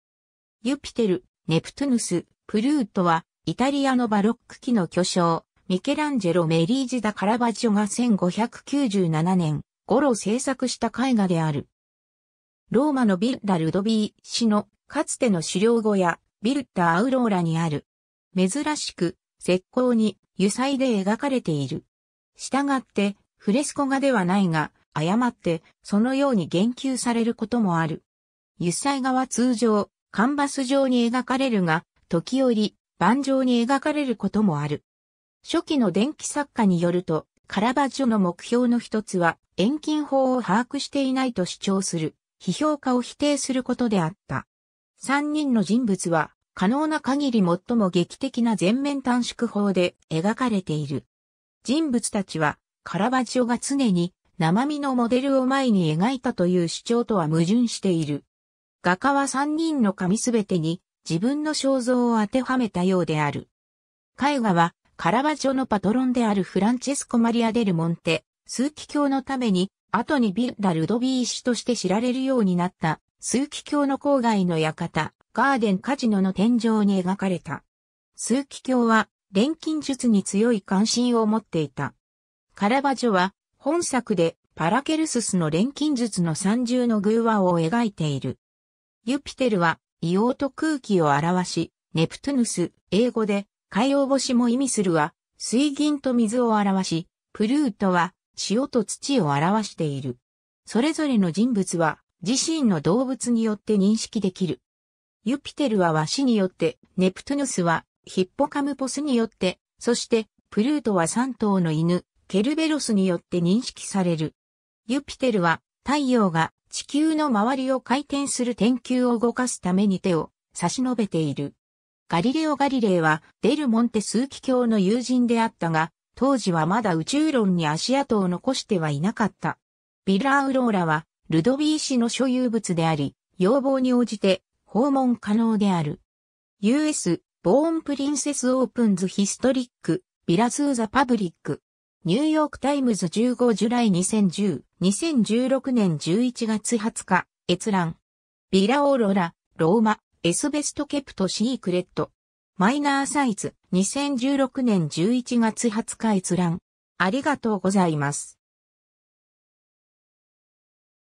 ユピテル、ネプトゥヌスとプルート
ミケランジェロ・メリージ・ダ・カラヴァッジオの作品
『ユピテル、ネプトゥヌス、プルート』（伊: Giove, Nettuno e Plutone）は、イタリアのバロック期の巨匠ミケランジェロ・メリージ・ダ・カラヴァッジョが1597年ごろ制作した絵画である。
ローマのヴィッラ・ルドヴィーシのかつての狩猟小屋、ヴィッラ・アウローラにある。
珍しく石膏に油彩で描かれている。
したがって、フレスコ画ではないが、誤って、そのように言及されることもある。
油彩画は通常、カンヴァス上に描かれるが、時折、板上に描かれることもある。
 初期の伝記作家によると、カラヴァッジョの目標の一つは、遠近法を把握していないと主張する批評家を否定することであった。
 三人の人物は、可能な限り最も劇的な前面短縮法 (foreshortening) で描かれている。
人物たちは、カラヴァッジョが常に生身のモデルを前に描いたという主張とは矛盾している。
画家は三人の神すべてに自分の肖像を当てはめたようである。
 絵画は、カラヴァッジョのパトロンであるフランチェスコ・マリア・デル・モンテ枢機卿のために、後にヴィッラ・ルドヴィーシとして知られるようになった、枢機卿の郊外の館「ガーデン・カジノ」の天井に描かれた。
枢機卿は錬金術に強い関心を持っていた。
カラヴァッジョは、本作でパラケルススの錬金術の三重の寓話を描いている。
ユピテル（英語で「木星」も意味する）は硫黄と空気を表し、ネプトゥヌス　(英語で「海王星」も意味する）は水銀と水を表し、プルート（英語で「冥王星」も意味する）は塩と土を表している。
それぞれの人物は自身の動物によって認識できる。
ユピテルは鷲によって、ネプトゥヌスはヒッポカムポスによって、そして、プルートは三頭の犬、ケルベロスによって認識される。
ユピテルは、太陽が地球の周りを回転する天球を動かすために手を差し伸べている。
ガリレオ・ガリレイはデル・モンテ枢機卿の友人であったが、当時はまだ宇宙論に足跡を残してはいなかった。
 ヴィッラ・アウローラはルドヴィーシの所有物であり、要望に応じて訪問可能である。
 ^ “U.S.-Born Princess Opens Historic Villa to the Public”. New York Times 15 July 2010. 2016年11月20日閲覧。
 ^ “Villa Aurora, Rome's best kept secret?”. Minor Sights. 2016年11月20日閲覧。